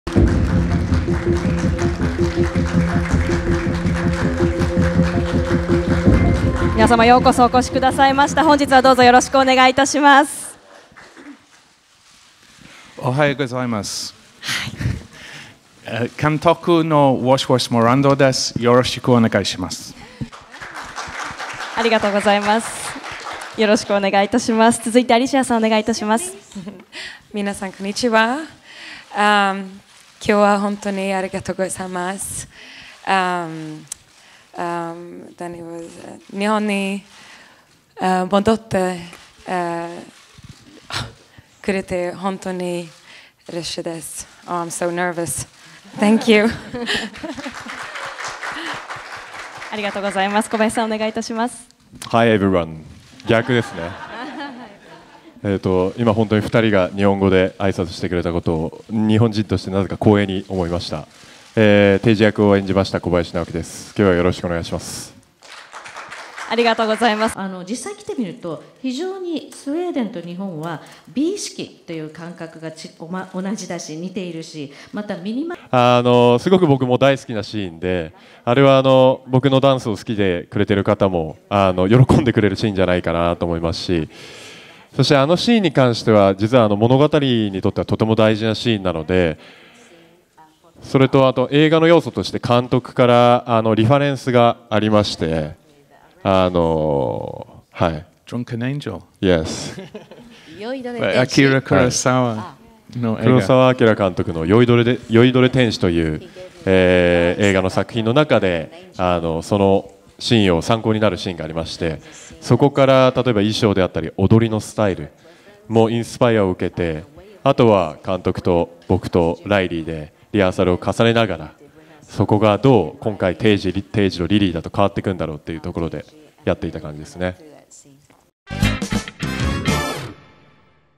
小林直己 Netflix映画でダンスにチャレンジ
10月29日（火）Netflix映画『アースクエイクバード』記者会見が行われ、監督のウォッシュ・ウェストモアランド、アリシア・ヴィキャンデル、小林直己が登場。